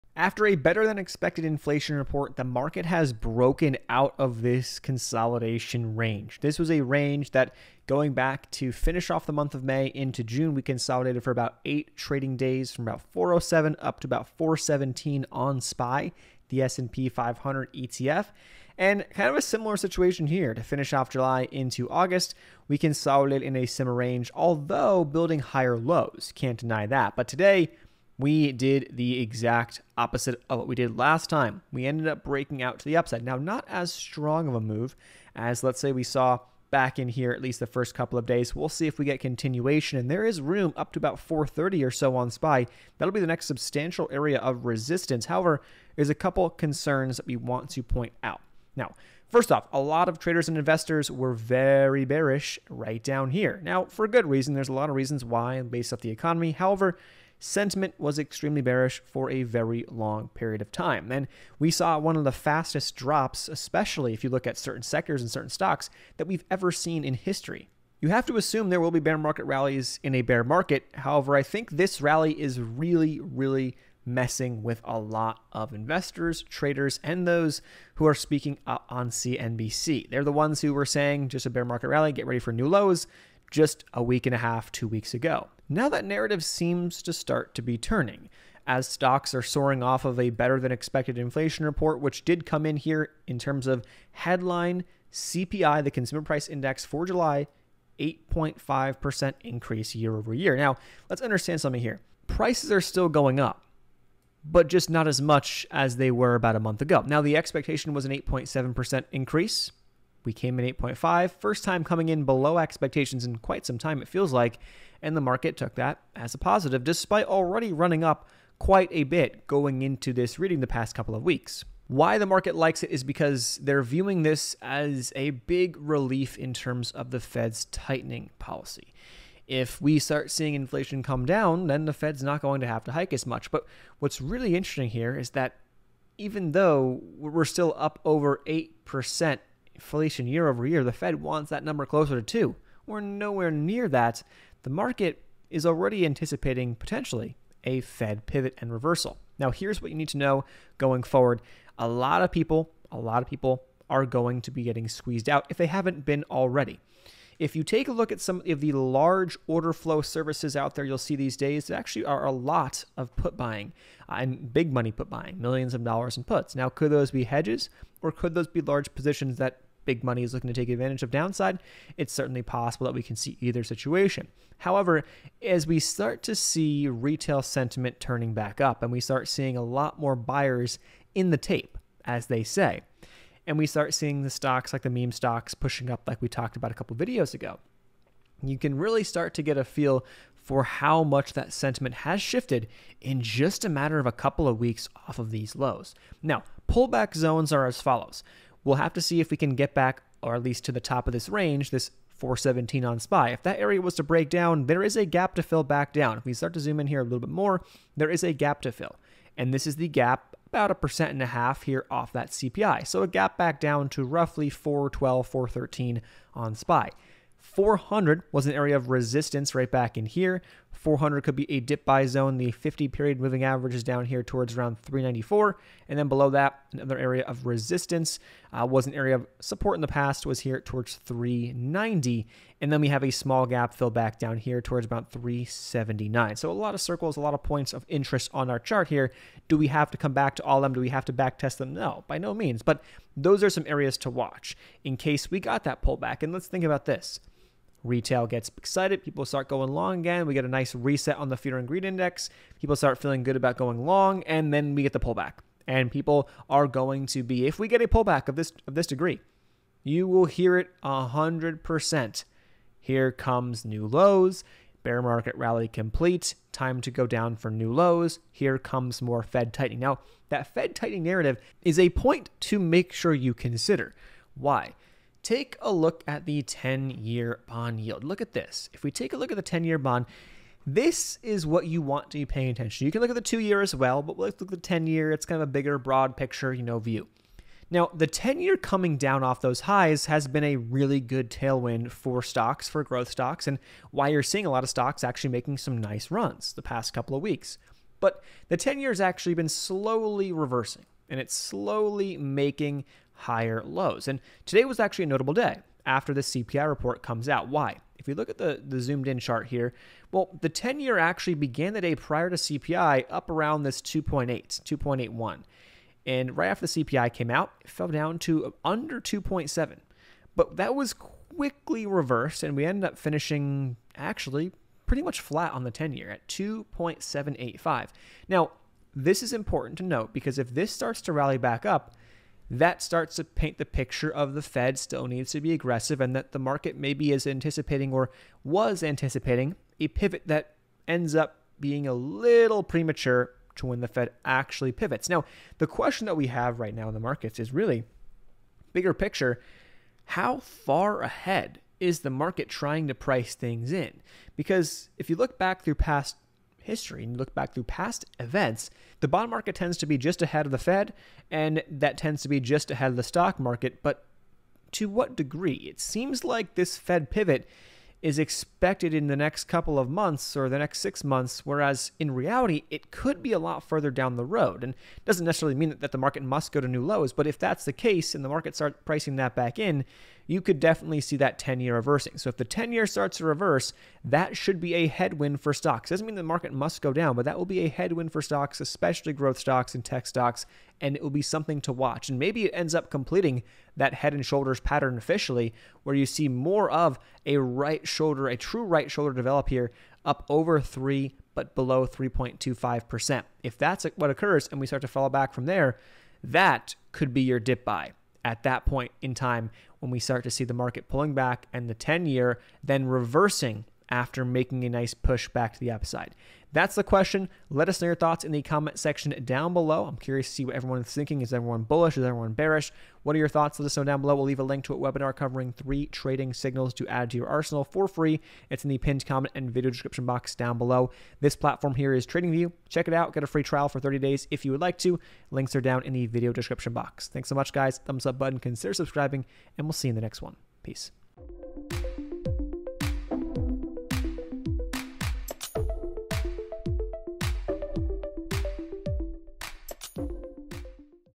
No One Understands Believed In This Rally, But The Bond Market Is Dropping Hints
In this video from Stock Trends we are covering an update on the stock market after July CPI data, the recent price action, price prediction, technical analysis and more! As always do your own due diligence before making any trades/investments and leave any questions and comments below in comments section!

                                  🔽 RESOURCES 🔽